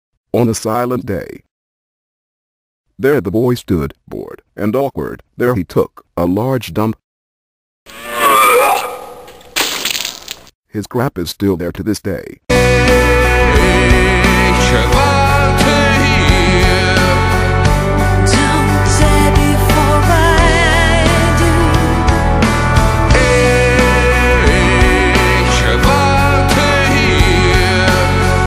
My first Paint Story video (Joke)
Try to understand the words, i made this with a computer voice generation program, so the voice is a little awkward